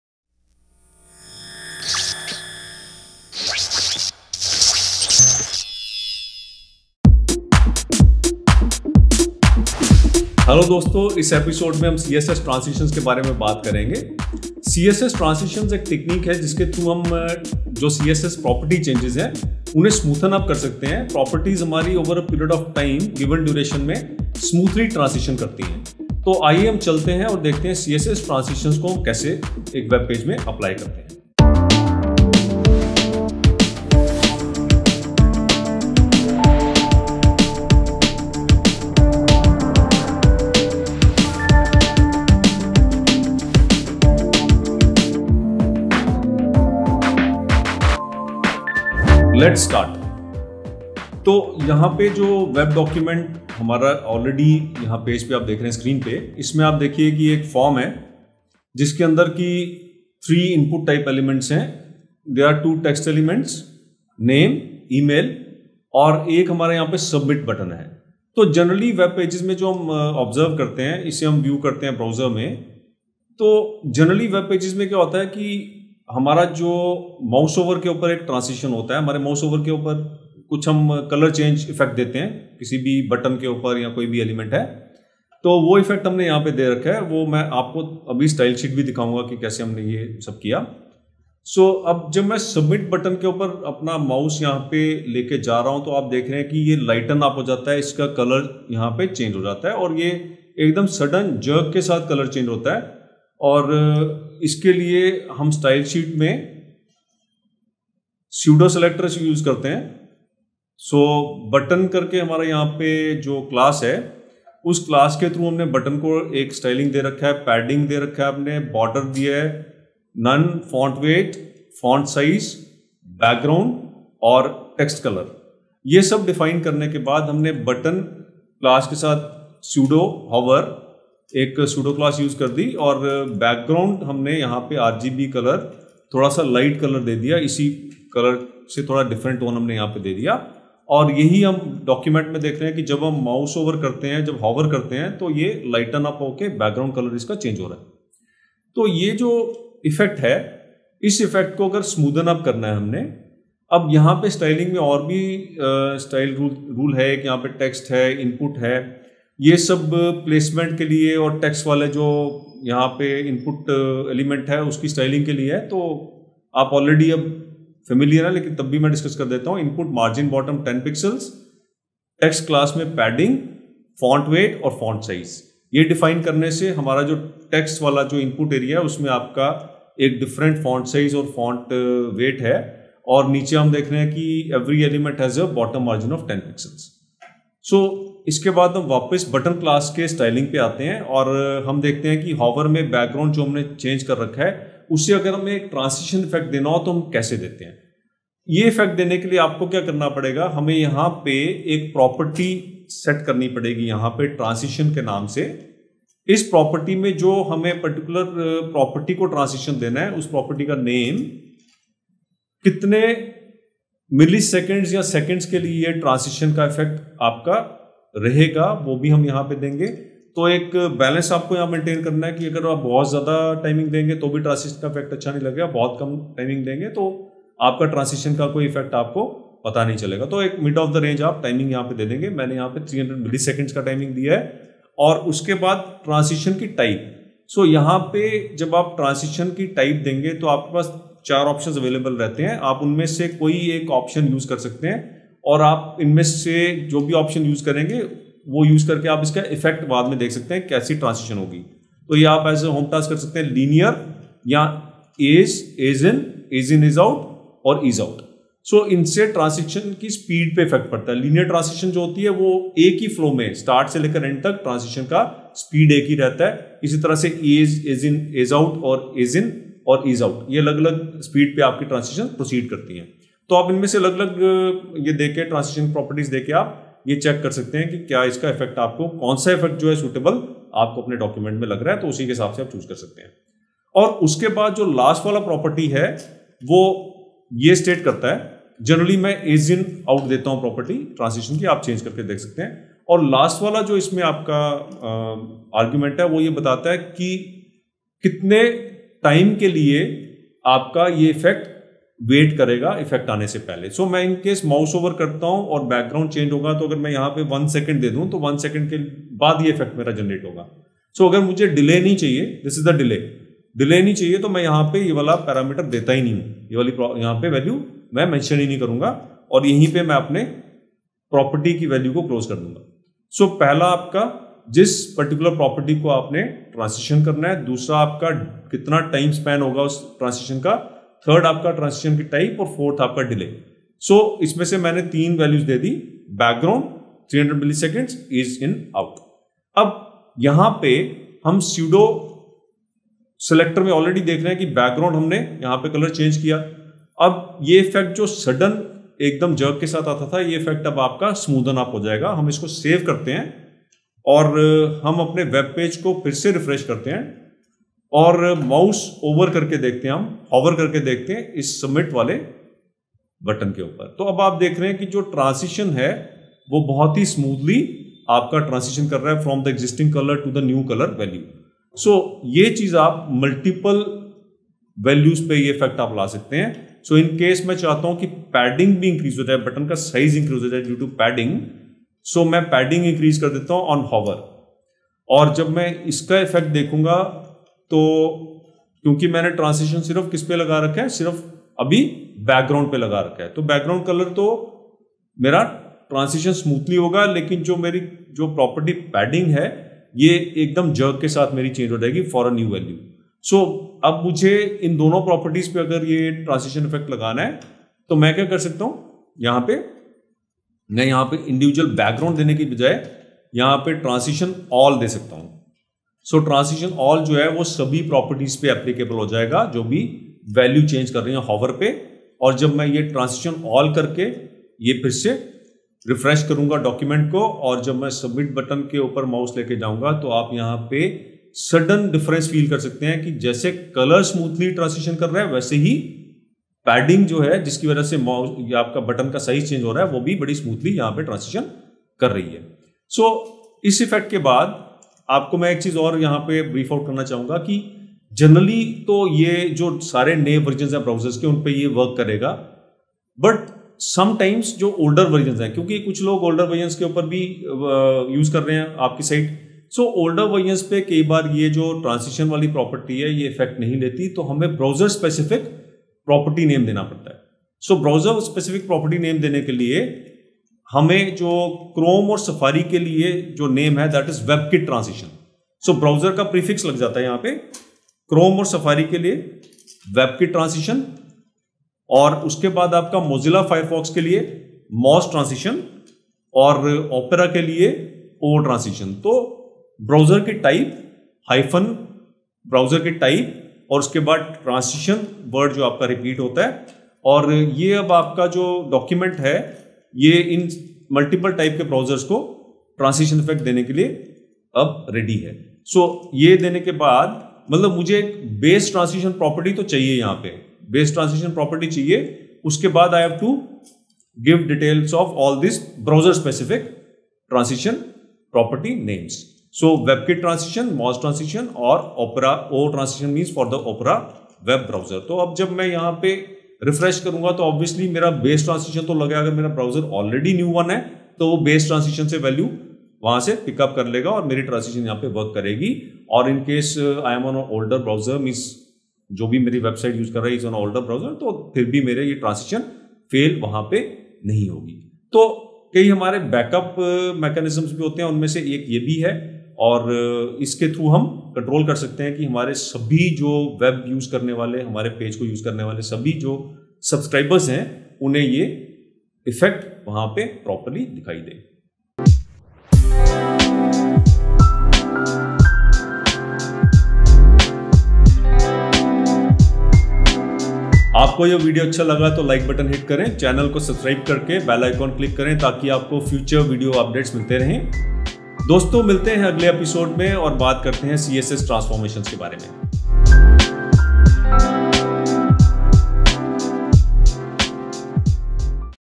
CSS Transitions | Episode 24 | CSS Tutorial in Hindi | CSS Beginner to Advanced 🔥
The video gives the concept of the CSS Transitions. It explains how we can smoothen the changes in html element properties by using the transition property of CSS.

SECTIONS
00:00 :Episode Intro
00:31 :Channel Banner
00:49 :Tutorial, CSS Transitions
09:21 :Code with Vivek
09:30 :Next Video Intro

This is one of the series on web development. Before you start working with CSS you should have a good working knowledge of HTML. For learning HTML please go through our series HTML Tutorial for Beginners